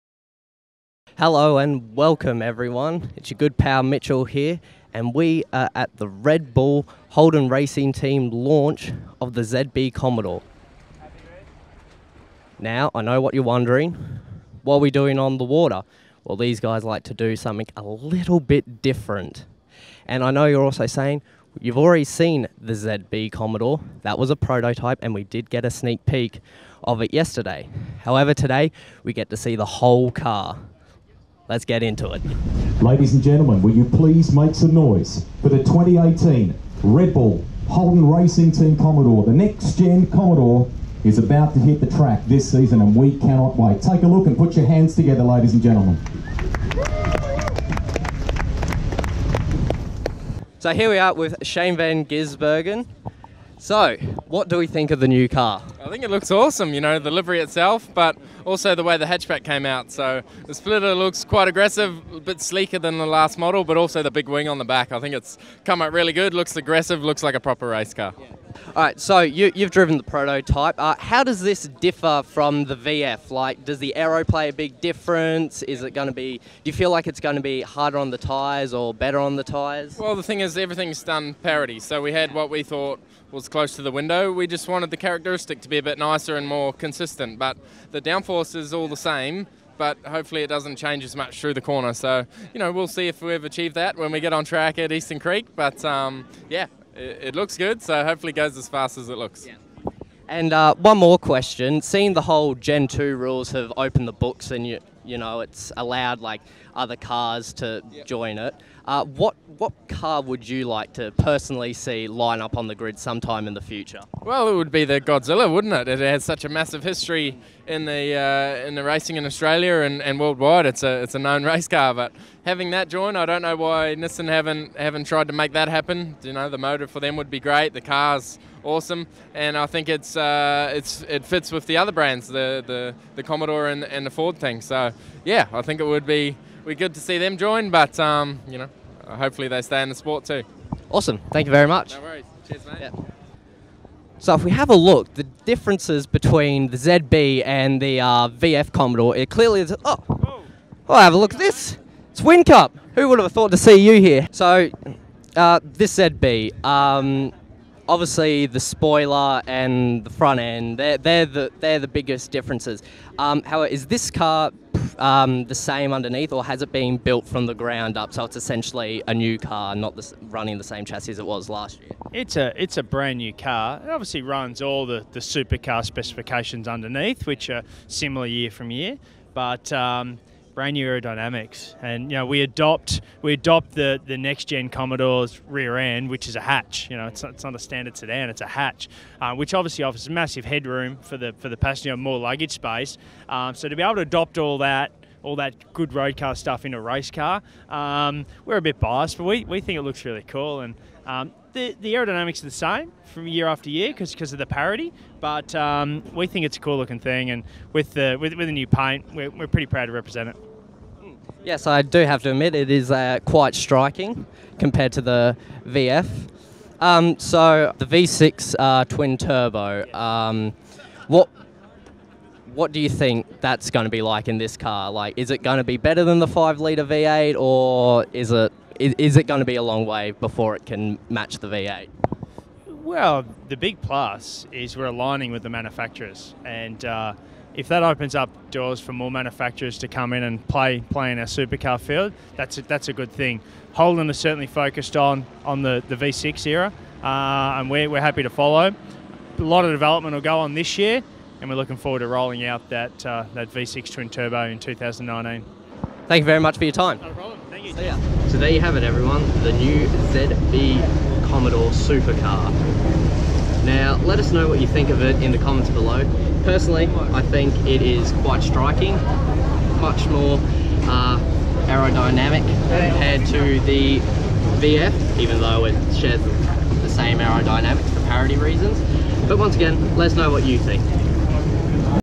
Mitch turns up the heat on Van Gisbergen and Whincup on the ZB Commodore Supercar
Oversteer - For car nuts and speed readers